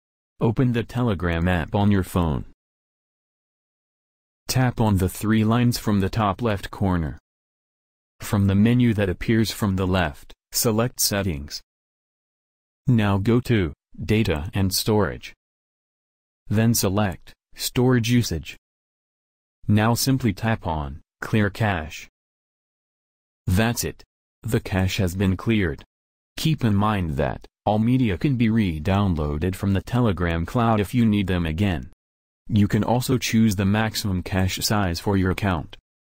How to Clear Cache on Telegram Messenger
In this short video I will show you, How to Clear Cache on Telegram Messenger.